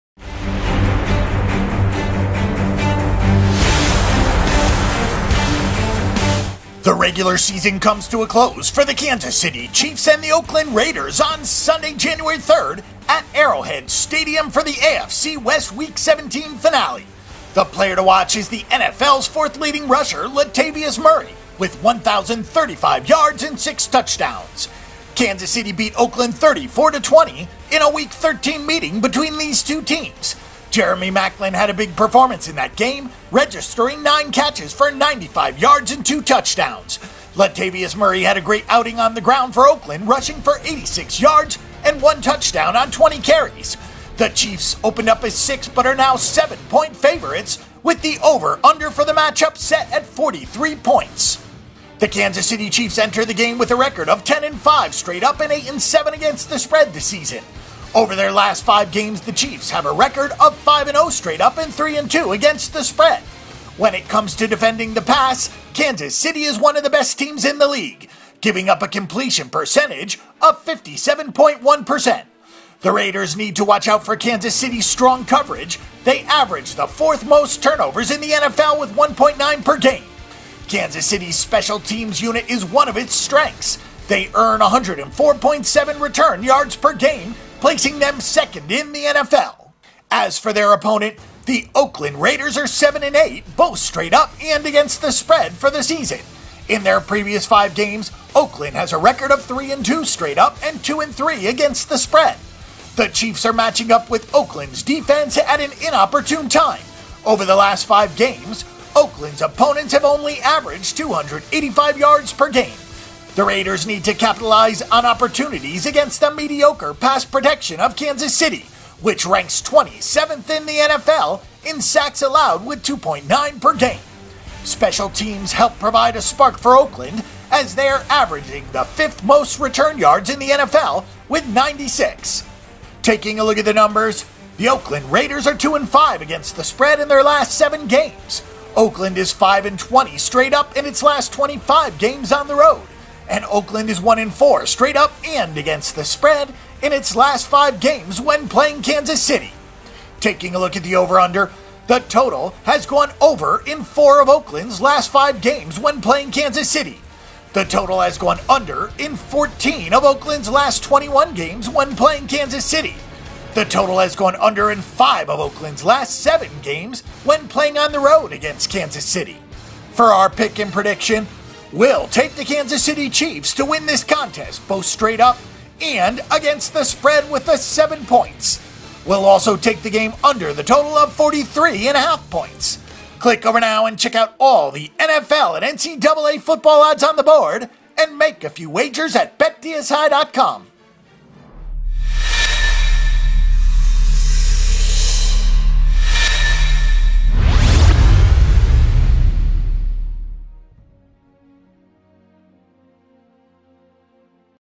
Oakland Raiders vs Kansas City Chiefs Odds | Free NFL Picks
Oakland Raiders vs Kansas City Chiefs Odds and Free NFL Picks

Oakland Raiders at Kansas City Chiefs – 4:25 pm (CBS)

The Kansas City Chiefs are favored at home on Sunday as they host the Oakland Raiders. The Chiefs have already clinched a playoff spot but they could win the AFC West if Denver happens to lose their final two games. The Chiefs are the hottest team in the NFL having won their last 9 games while the Raiders come into this game at 7-8. These teams met in early October in Oakland and the Chiefs won 34-20.

Week 17 NFL Odds

Oakland Raiders +7

Kansas City Chiefs -7

Total 43.5

ATS Stats

The teams have split their last ten both straight up and against the spread with the last four going over the total. Oakland has won six of the past ten at Kansas City and they are 7-3 ATS with five of the last seven going under.

NFL Picks

This is a tough call as the Chiefs may not have any incentive if the Broncos have already clinched the AFC West. I will take the points with the Raiders in this one.